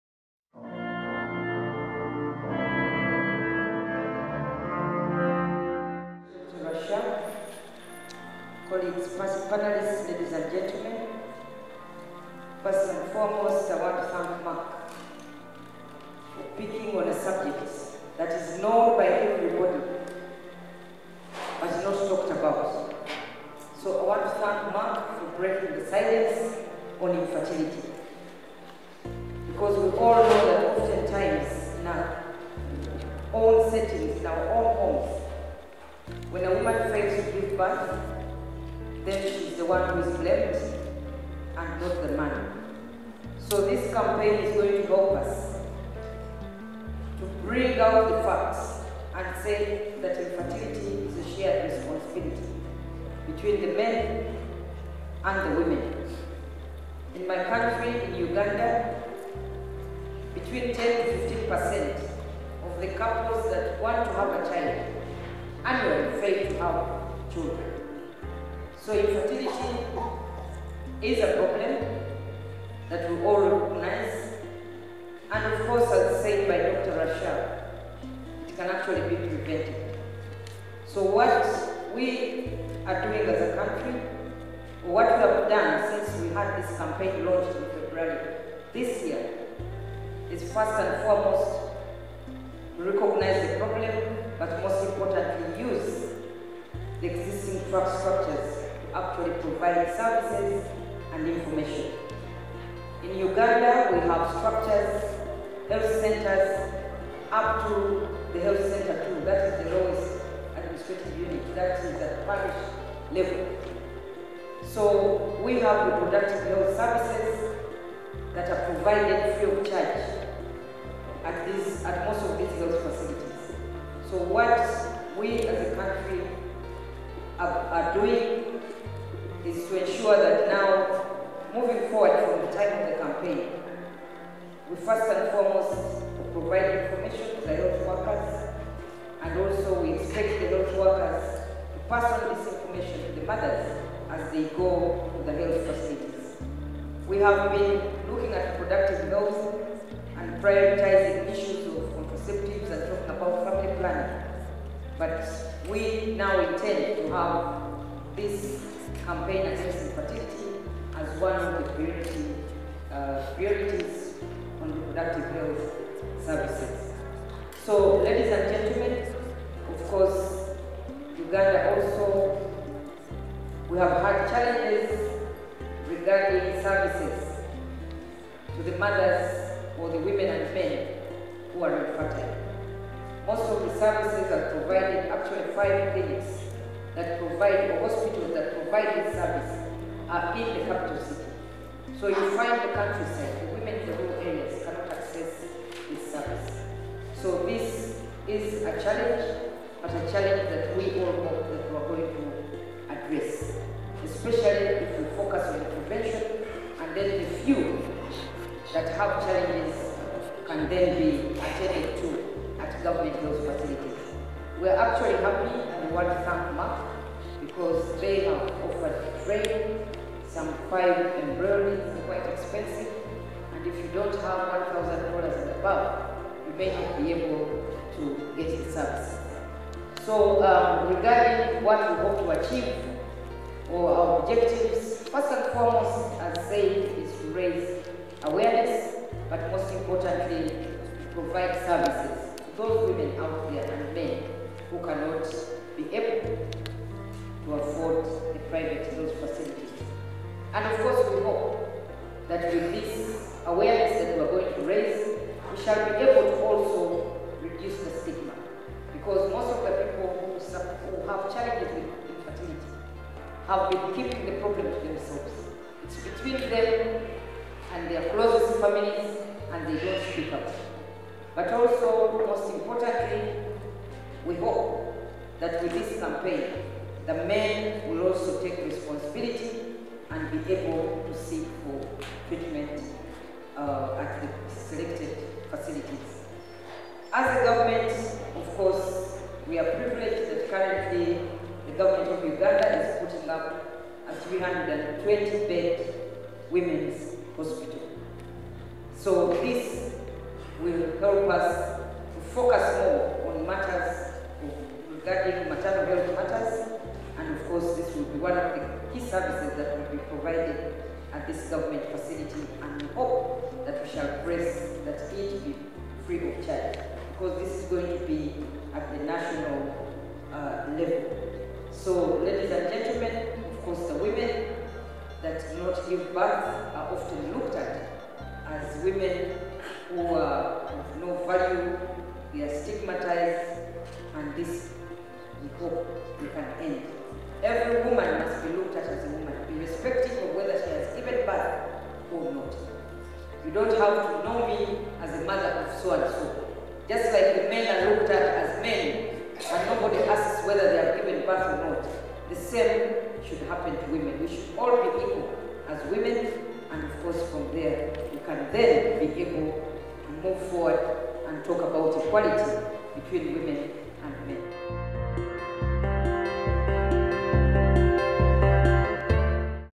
Merck More Than A Mother Campaign - Pan African Launch -H.E.Hon. Sarah Opendi
Merck More Than A Mother Campaign - Pan African Launch was held in April, 2016, in Cairo, Egypt.

During the Event Dr. Frank Stangenberg-Haverkamp - Merck Family Member and Chair Executive Board and Family Board of Merck, has been awarded African Alliances HE For SHE Award For Women Empowerment and Merck More Than a Mother A Mother.

High-Level Panel of Eminent Persons attended the Pan African More Than a Mother Campaign Launch:

Prof. Santos Nicolau :- Dean of Augustine University, Angola
Dr. James Olobo-Lalobo:-  Vice-President of Africa Fertility Society

H.E. Amb. Mona Omar:- Chair of International Relations Committee National Council of Women – Egypt
Prof. Oladapo Ashiru :- President of Africa Fertility Society
Hon. Joyce Lay:-  Member of Parliament, Taita-Taveta County, Kenya
Hon. Dr. Lanre Tejuoso:- Chairman Senate Committee on Health, Senate of the Federal Republic of Nigeria
H.E. Julia Duncan-Cassell:- Minister of Gender, Liberia
Prof. Dr. Frank Stangenberg-Haverkamp :- Merck Family Member and Chair Executive Board and Family Board of Merck
H.E. Hon Sarah Opendi :- Minister of State of Health of Uganda
Hon. Qedani Dorothy Mahlangu:- MEC Health Gauteng Government, South Africa
Hon. Betty Amongi:- Chair of Uganda Women Parliamentarian Association
H.E. Hon. Jean Kalilani:- Minister of Gender, Children, Disability and Social Welfare of Malawi
Hon. Maria Angelina Enoque:- ember of Parliament of Mozambique
Dr. Rasha Kelej :- Chief Social Officer-Vice President, Merck Healthcare, Chair of Health Committee, BPW-Egypt and African Alliance for Women Empowerment.

Hon. Laurette Andree De Mel Nee Yace :- Member of Parliament of Cote d’ Ivoire
Hon. Susan Lyimo:- Vice Chair of Women’s Parliamentary Caucus of Tanzania